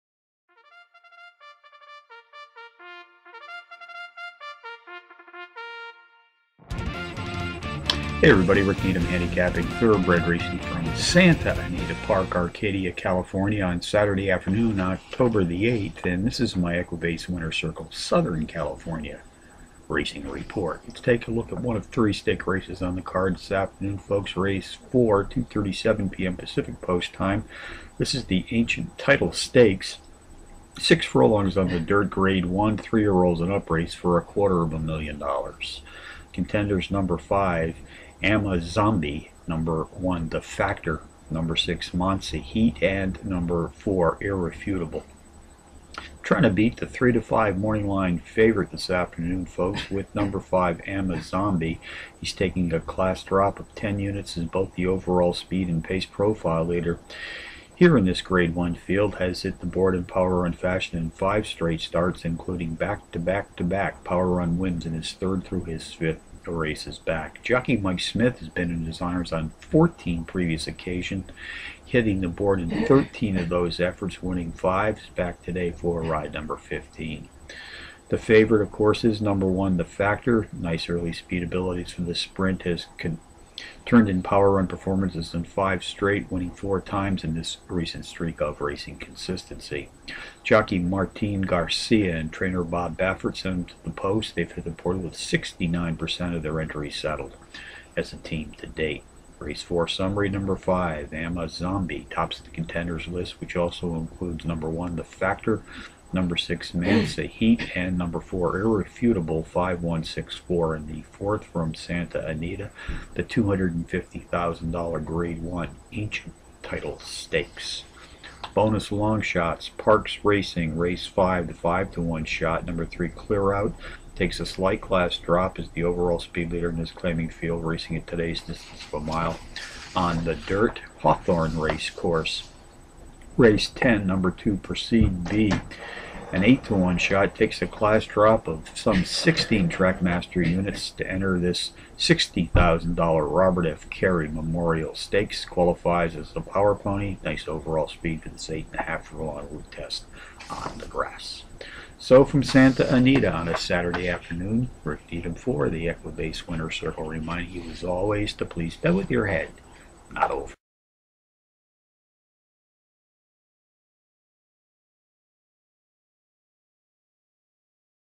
Race #4 - Santa Anita - Ancient Title Stakes - 10/08/2011
Race #4 - Santa Anita Park - $250,000.00 Grade I Ancient Title Stakes - Saturday October 8, 2011 + bonus longshots